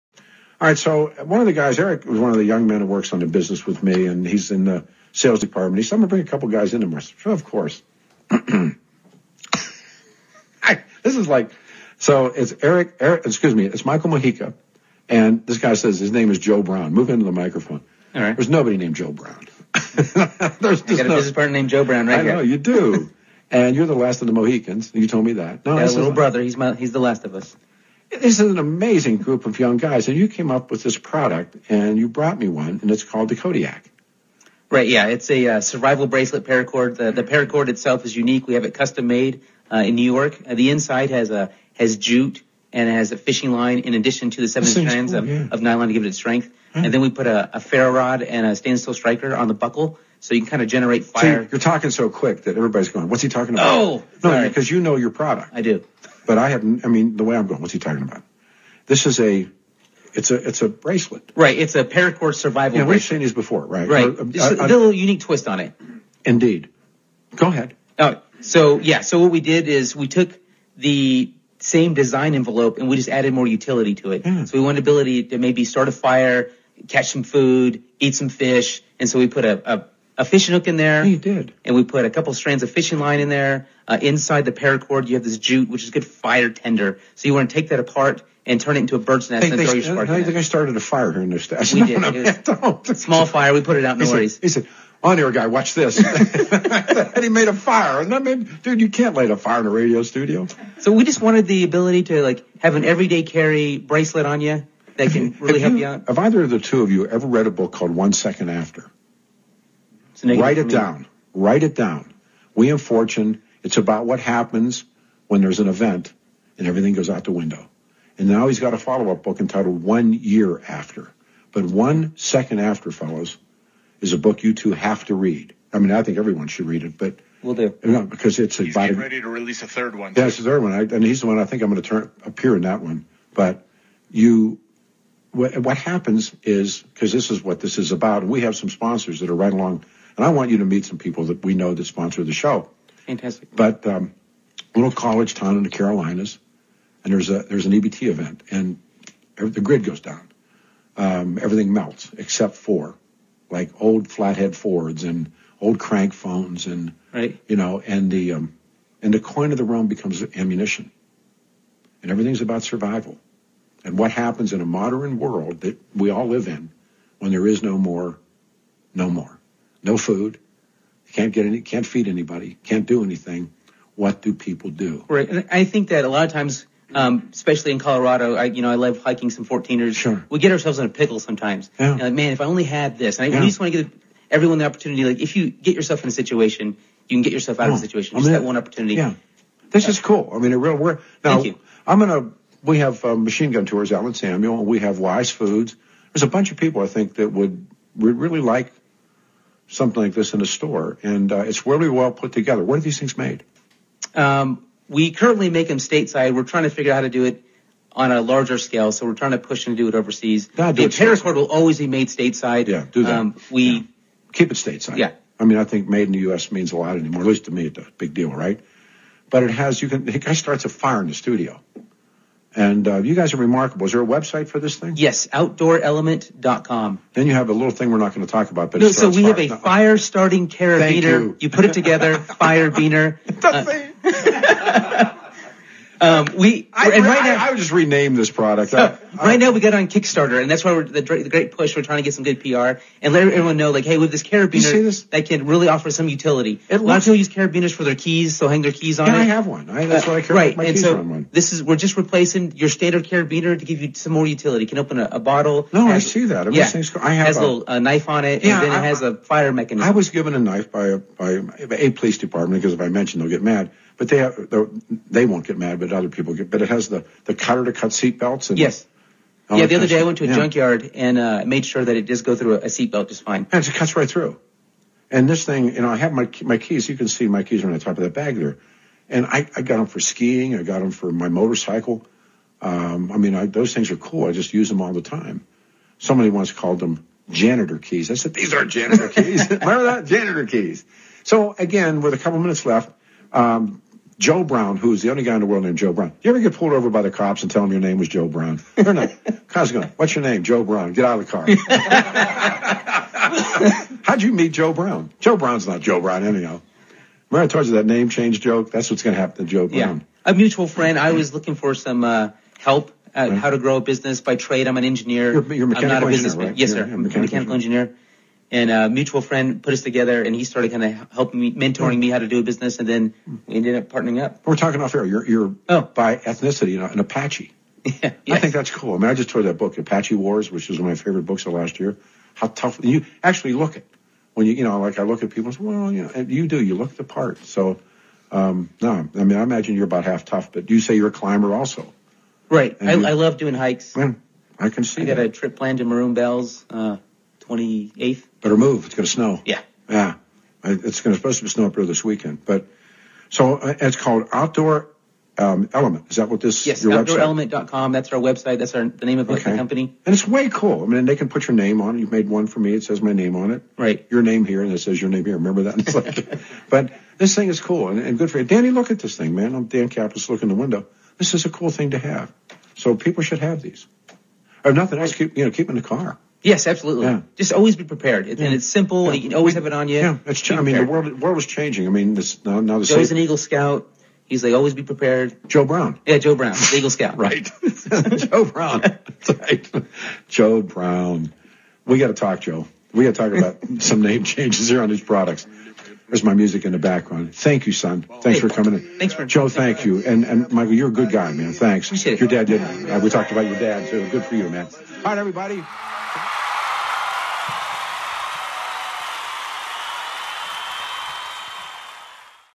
710 KNUS Peter Boyles with Outdoor Element Kodiak Firebiner 22 Sept 2016
Peter Boyles of 710 KNUS spent some time with a couple of partner of outdoor element. We talked about the Kodiak Paracord Survival Braid and the fire starting carabiner, the firebiner. Peter is not one to shy away from his opinions...he liked both products and Mike and Joe had a great time in the studio. The firebiner is currenlty available on Kickstarter